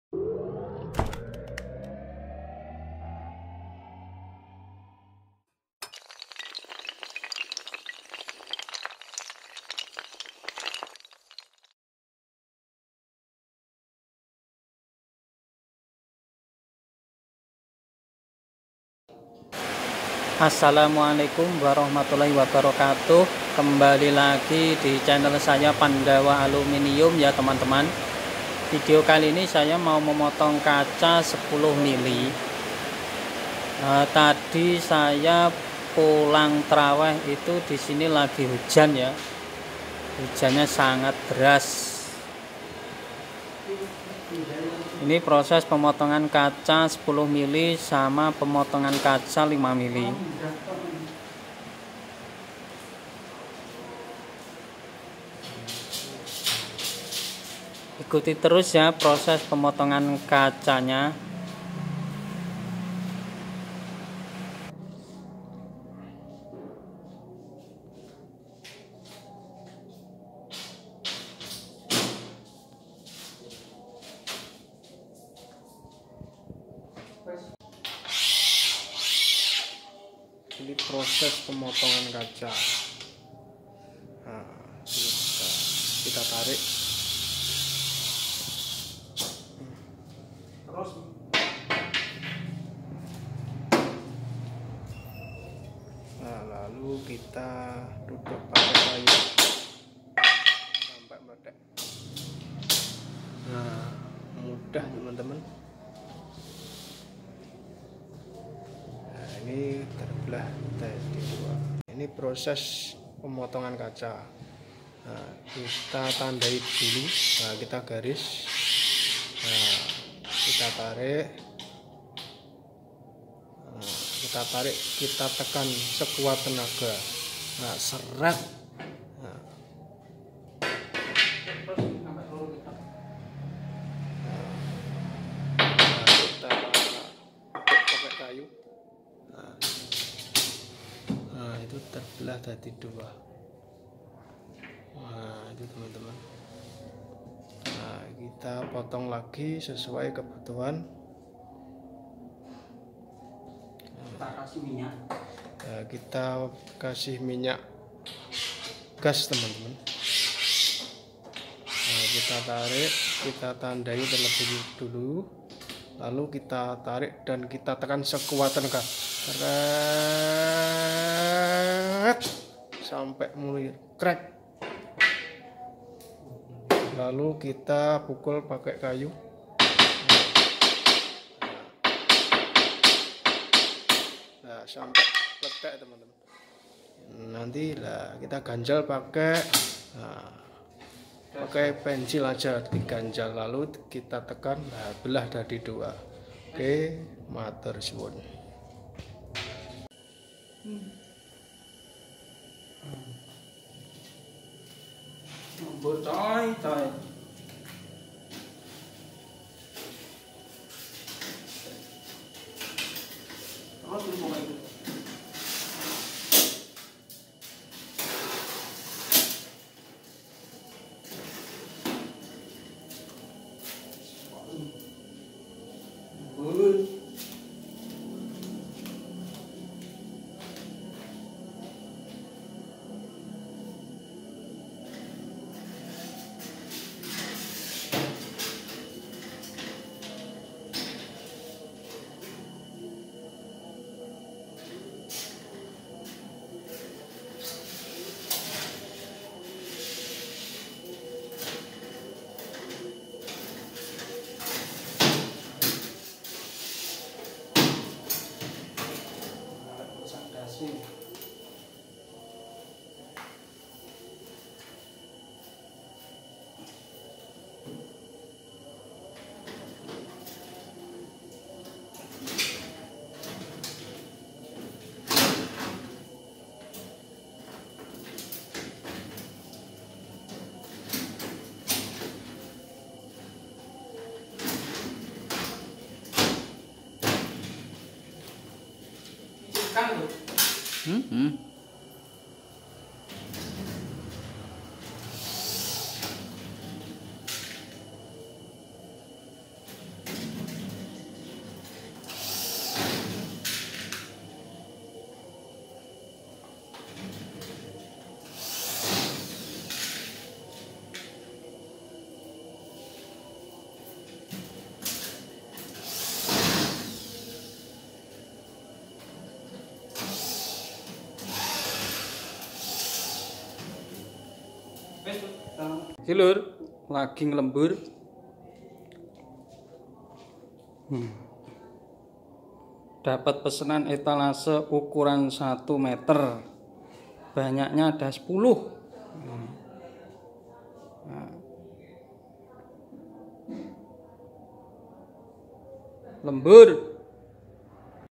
pemotongan kaca buat ngisi etalase aluminium
malam ini saya lagi lembur potong kaca 10 ml sm potong kaca 5 ml teman teman.buat persiapan pasang jendala sam buat ngisi etalase aluminium ukuran 1meter.